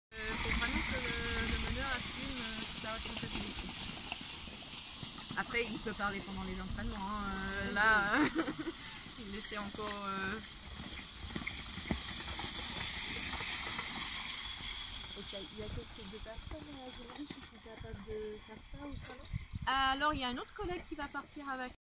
Entrainement de Rainette et Vénus, les mules poitevines au Salon de l'Agriculture
Vénus et Rainette, mules poitevines de 9 et 13 ans, s'entraînent à l'Asinerie du Baudet du Poitou, à Dampierre-sur-Boutonne (17). Elles sont sur le Salon de l'Agriculture du 24 février au 5 mars 2018.